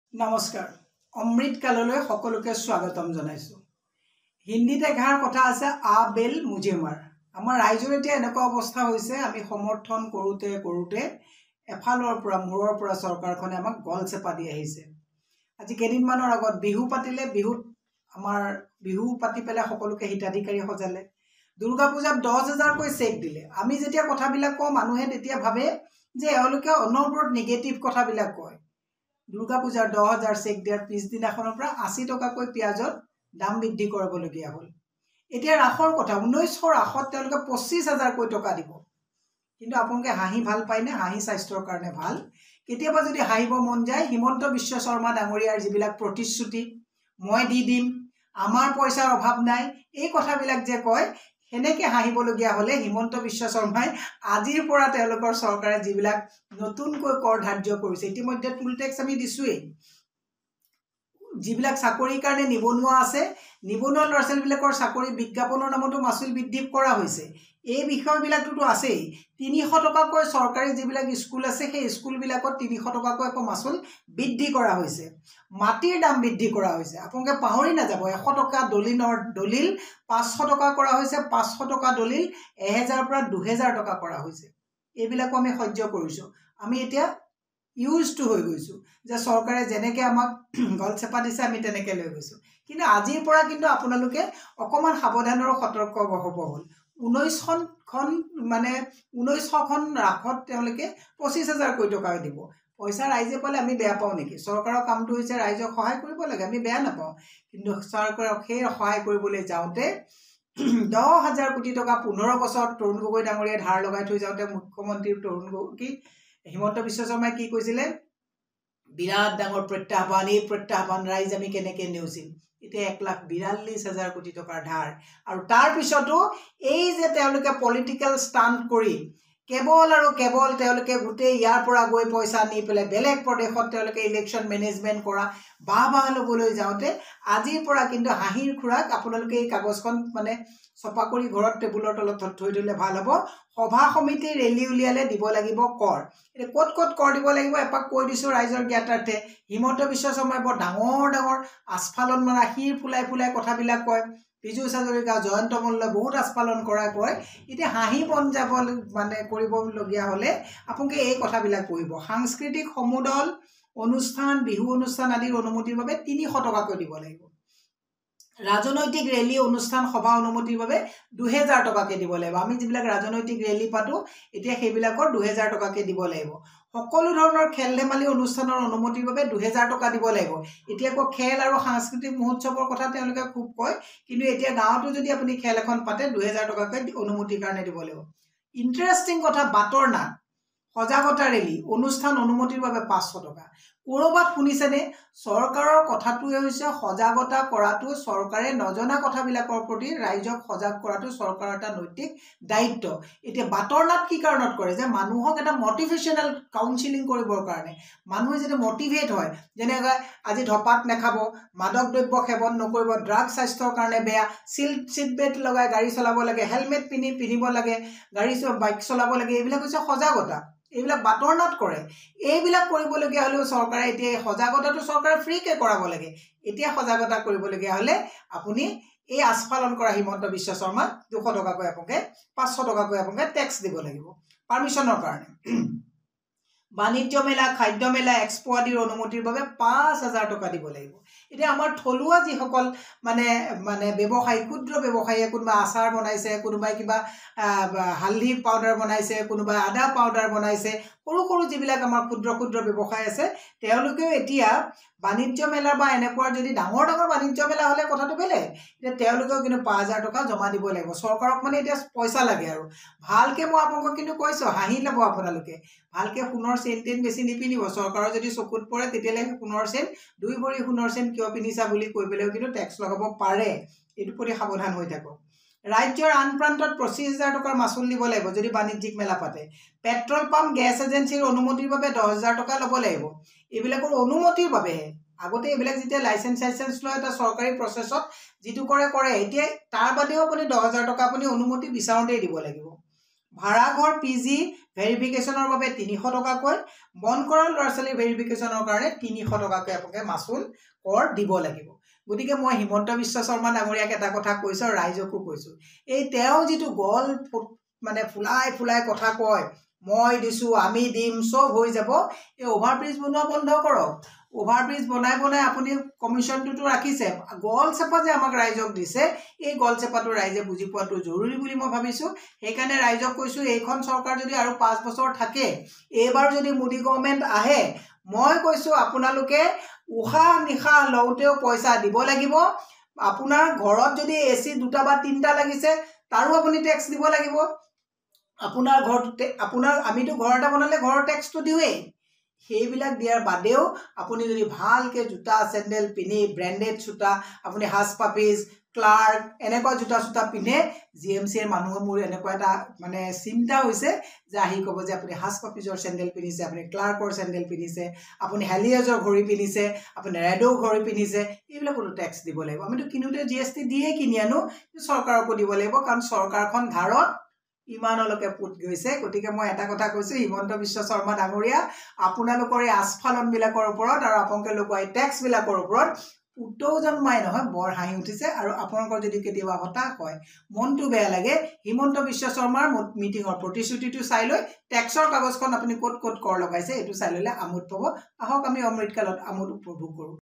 Assam Govt New Tax Policy : বিহু পাতিলেও এতিয়া ৰাজ্যত দিব লাগিব কৰ | Mira Borthakur | N18V
Assam Govt New Tax Policy | বিহু পাতিলেও এতিয়া ৰাজ্যত দিব লাগিব কৰ। বিহু পাতিবলৈ অনুমতিৰ নামত ৩০০টকা মাচুল। উল্লেখ্য যে এই সকলো কৰ অসম আৰক্ষীৰ সেৱা প’ৰ্টেলৰ জৰিয়তে দিব লাগিব মাচুল।
পূৰ্বে ইয়াৰ অধিকাংশ কৰ চৰকাৰক আদায় দিয়া হৈছিল যদিও এতিয়া পুনৰ সংশোধন আৰু বৃদ্ধি কৰা হৈছে।